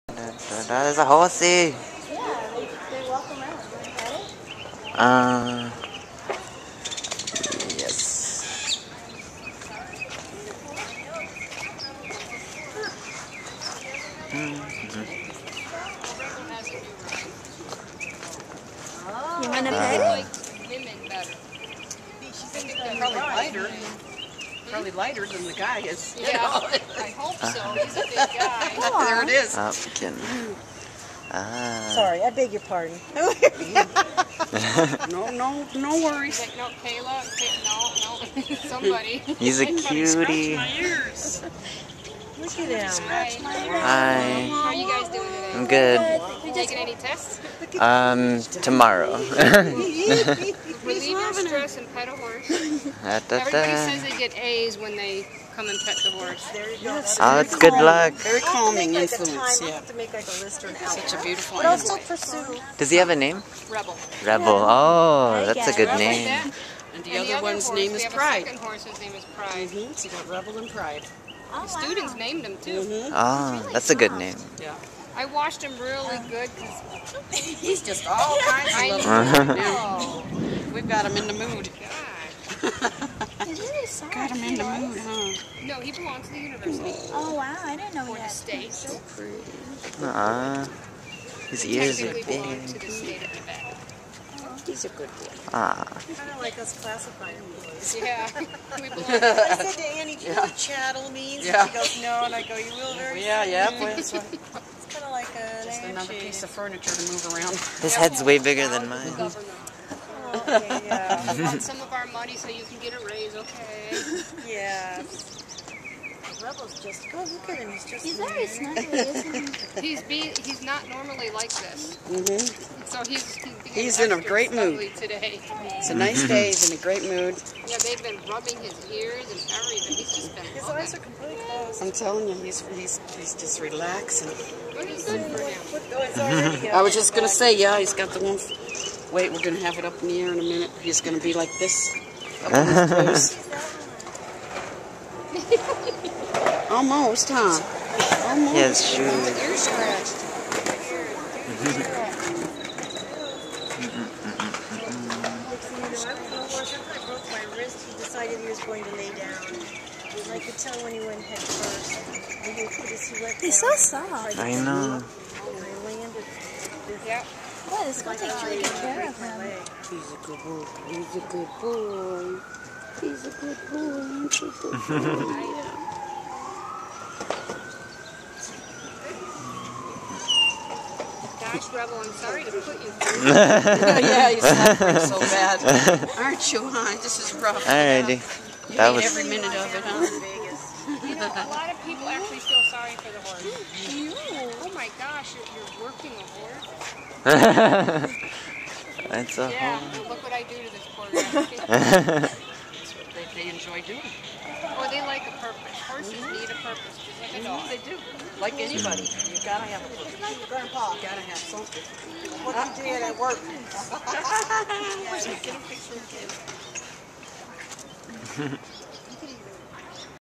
Petting A Horse At UNLV
Met campus security with a horse named "Rebel."